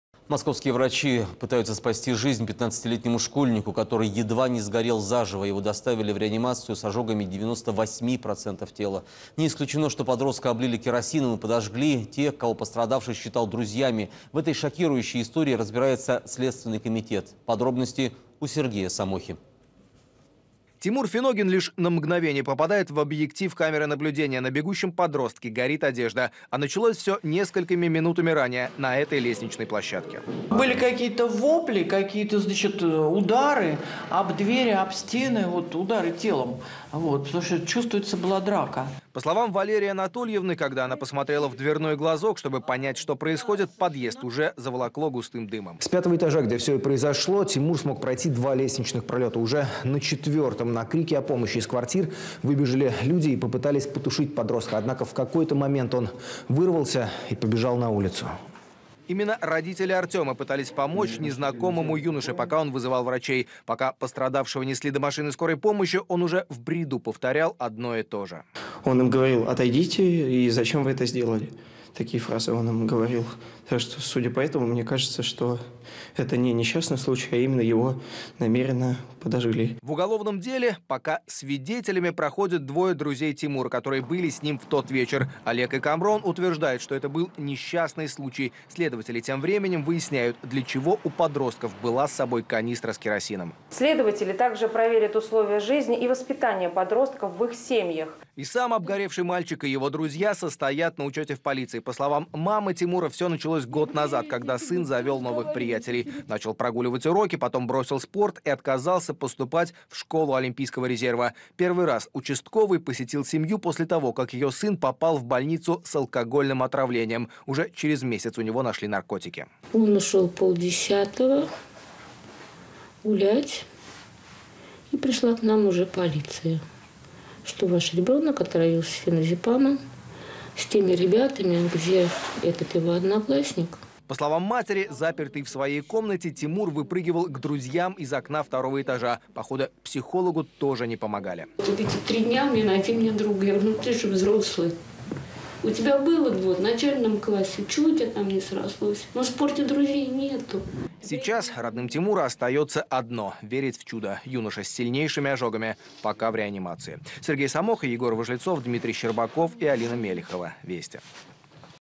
Облитый керосином обгоревший подросток бросил школу олимпийского резерва
Московские врачи спасают жизнь 15-летнему школьнику, который едва не сгорел заживо. Его доставили в реанимацию с ожогами 98 процентов тела. Не исключено, что подростка облили керосином и подожгли те, кого пострадавший считал друзьями. В этой шокирующей истории разбирается Следственный комитет.